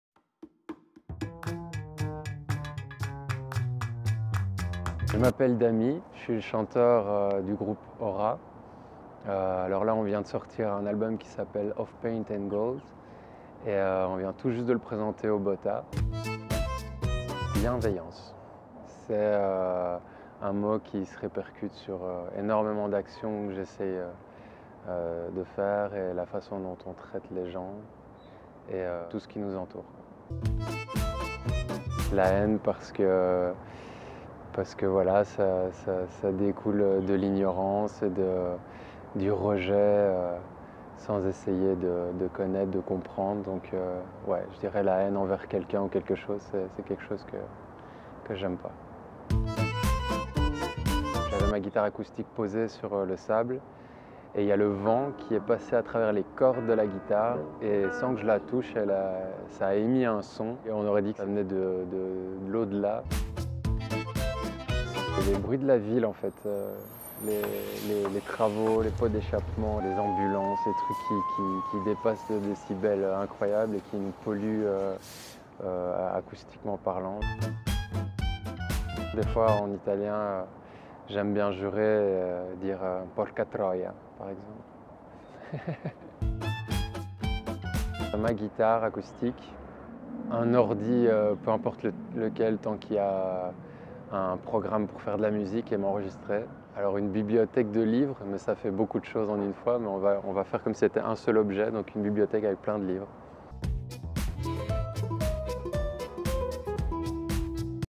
Interview express de O.R.A
O.R.A. (pour Organic Random Atmosphere) est un groupe electro rock alternatif. Le quintet s’est créé autour de l’amour qu’ils ont pour la musique intense, inspirante, spirituelle et rêveuse qui vous plonge dans une introspection et vous fait vibrer au plus profond. Leur musique oscille entre textures électroniques retenues et instruments acoustiques puissants, le tout porté par la voix envoûtante du chanteur : Dami Perri. C’est avec lui que nous nous sommes entretenus un long moment.